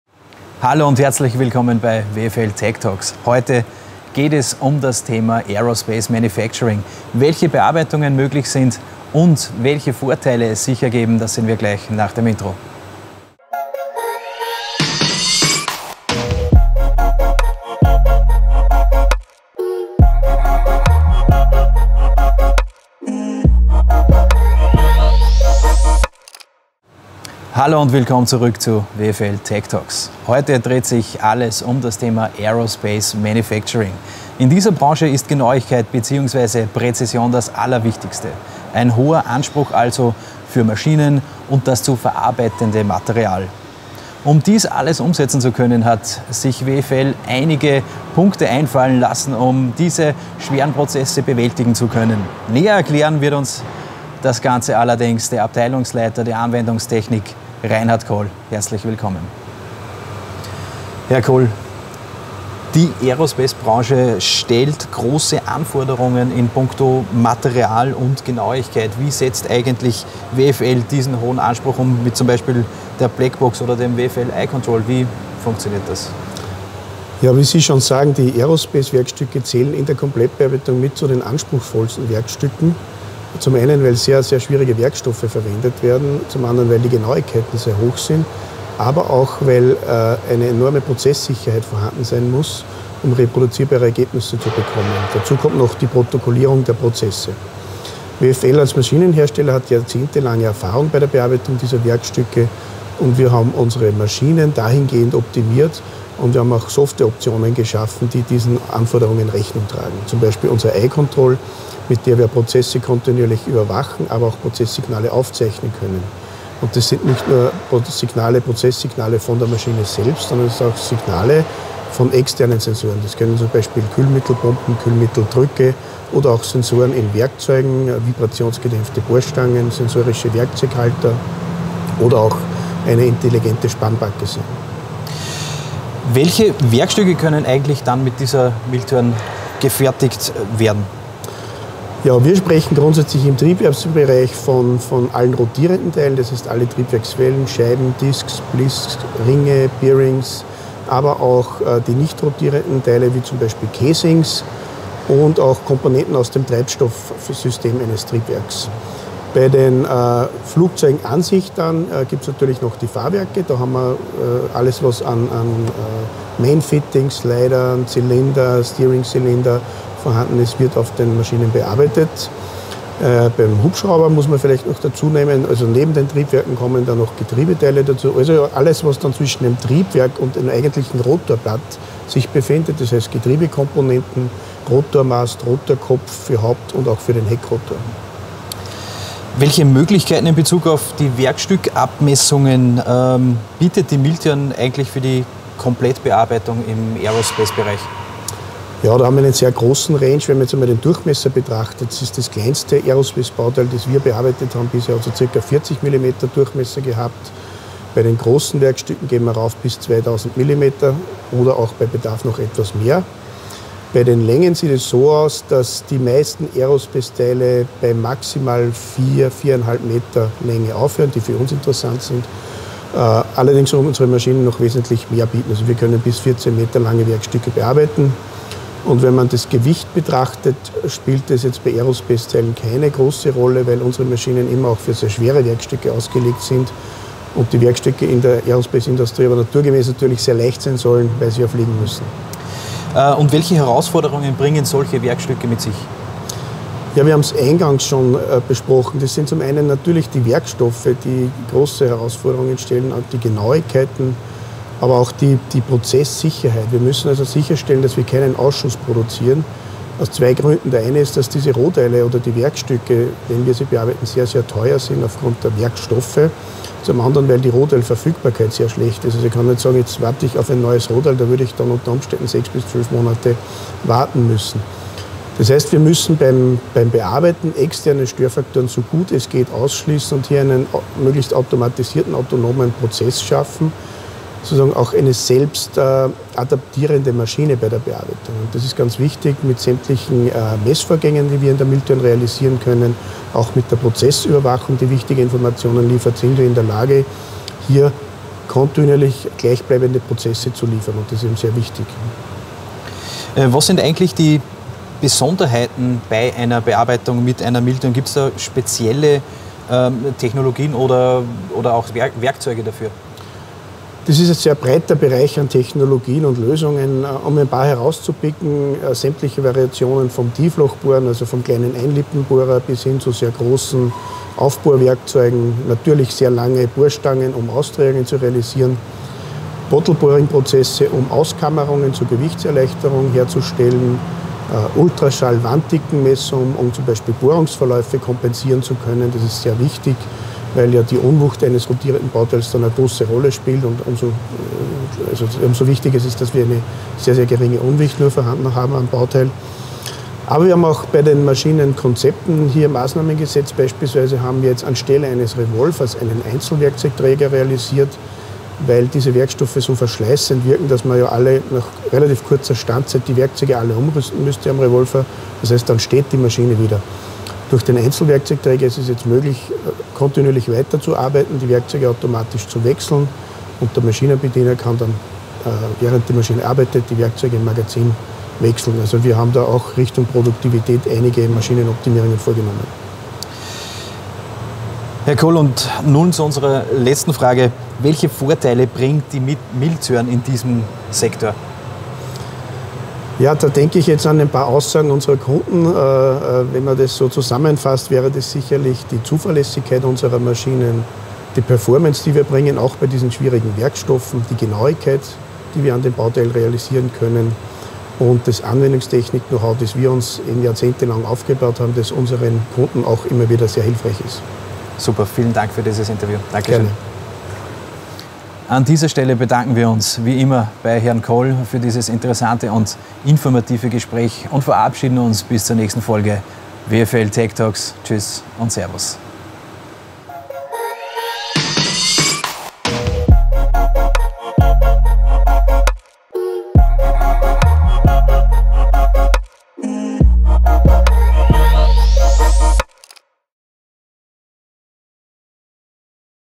WFL TECtalks Epiode 6: Aerospace Manufacturing
Small batches of complex workpieces made from high-temperature-resistant metals are standard in the aerospace industry. Maximum precision and reliable machining of complex geometries are factors that are particularly in demand here. The various technological challenges involved in this machining process usually require solutions that have been specially adapted. The objective here is to ensure that machining is economical, therefore the advantages of complete machining are particularly illustrated in this sector.